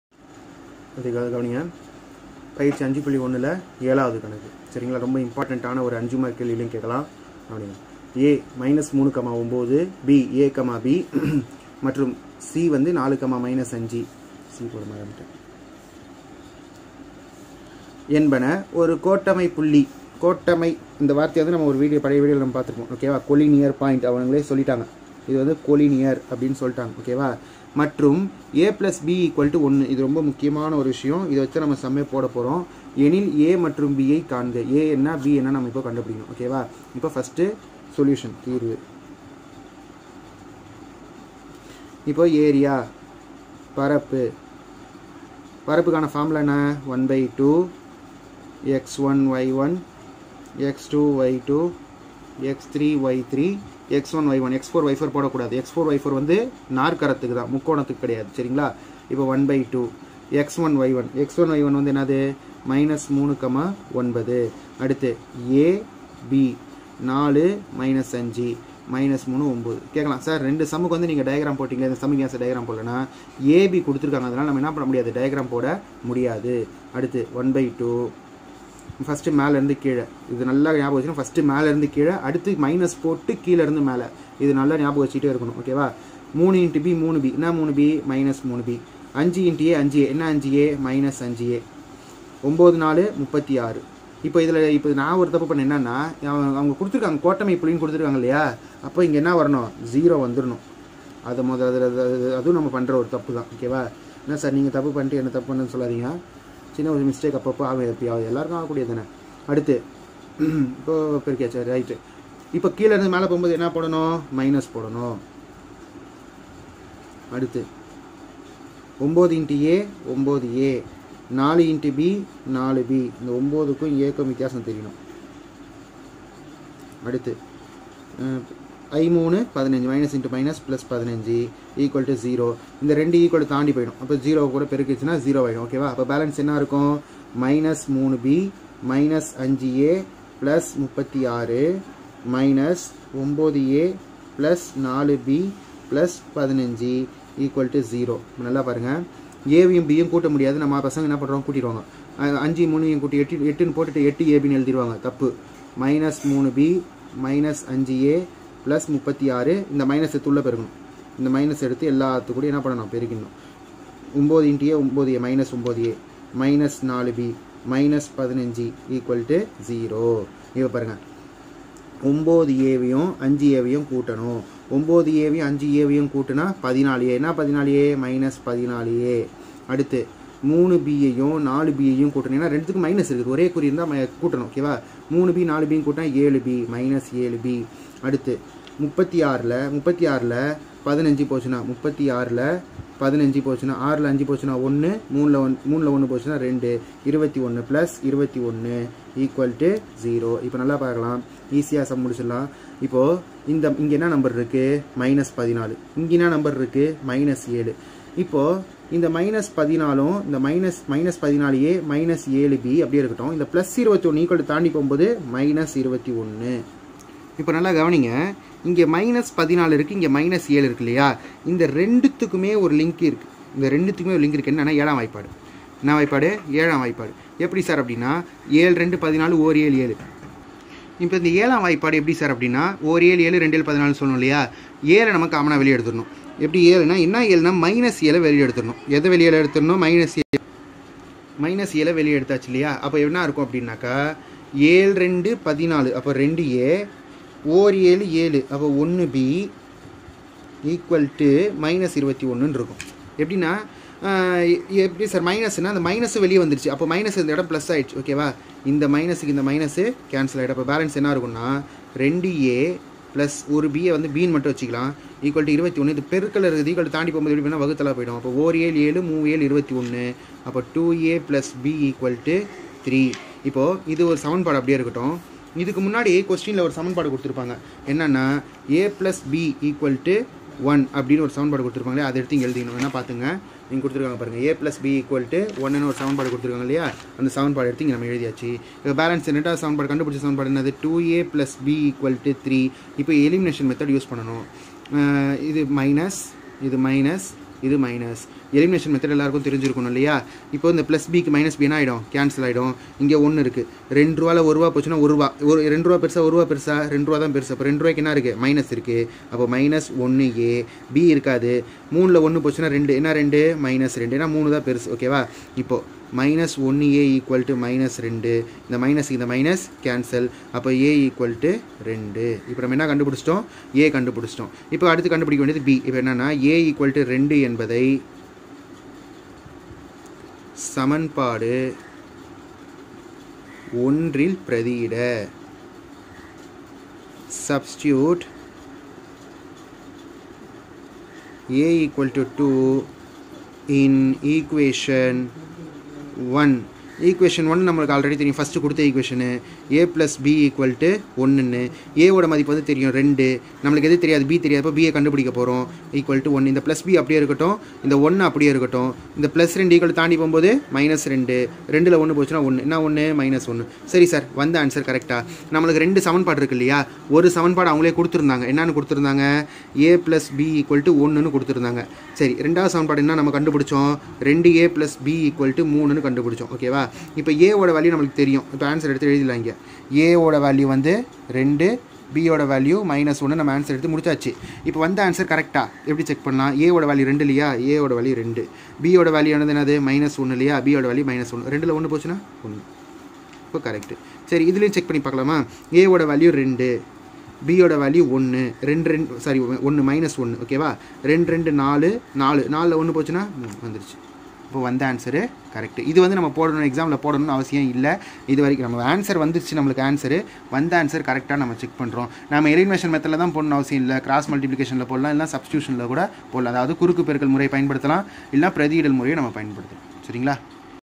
IF A B C are collinear and a+b=1 then find the value a and b. SSLC MATHS/TN/ EX : 5.1 (7)th SUM
maths sharuk basha
GUMMIDIPOONDI
THIRUVALLUR DISTRICT